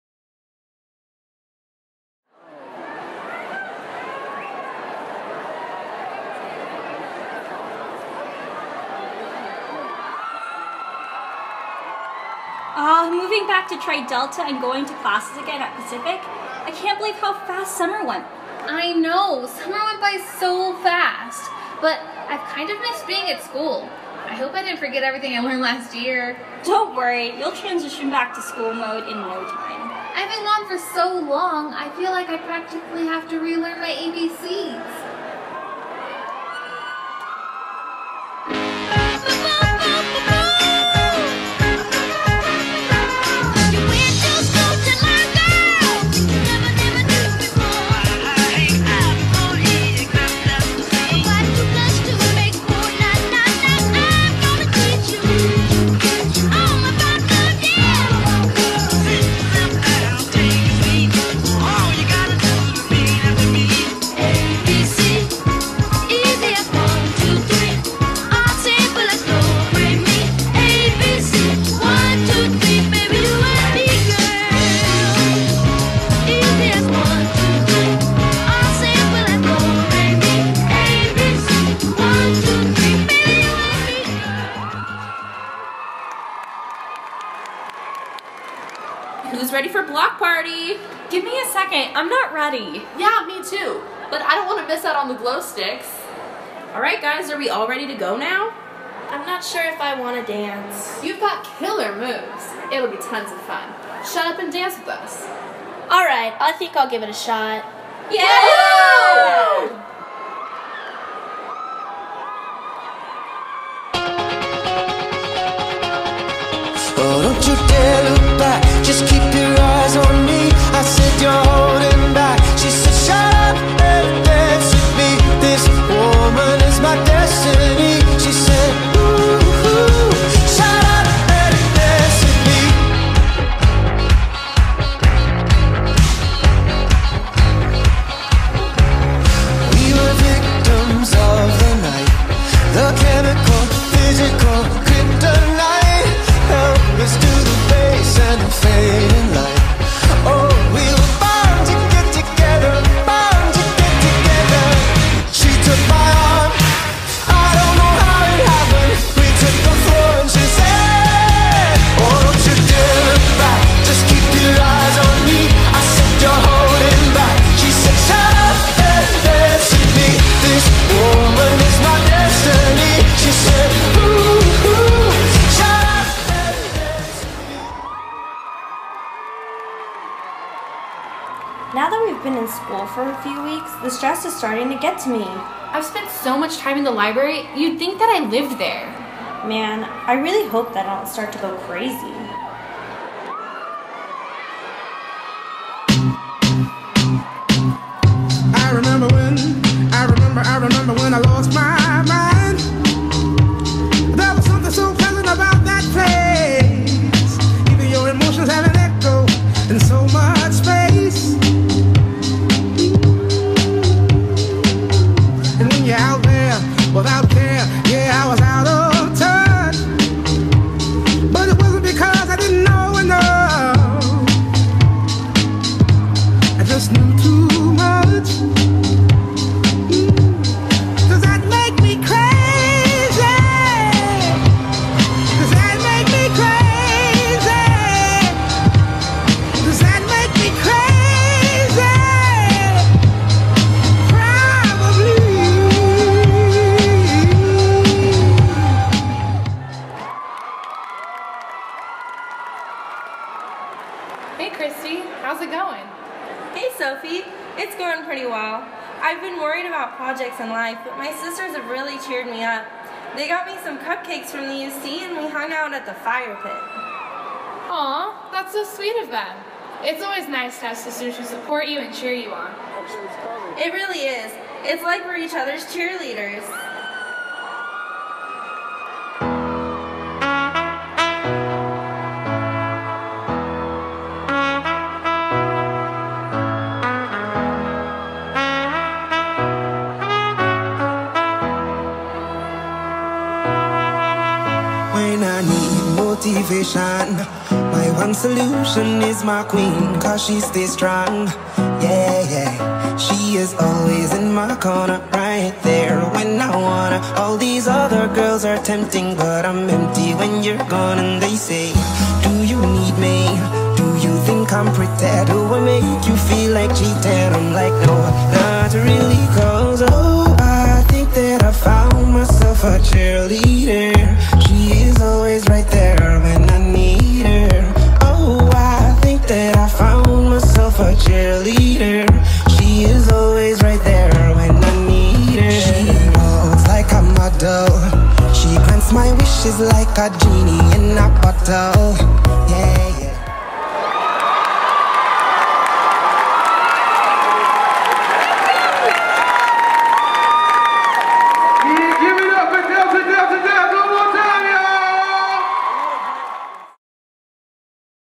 Tri Delta
University of the Pacific Homecoming Tiger Rally October 16th, 2015. Lip sync performance by Delta Delta Delta.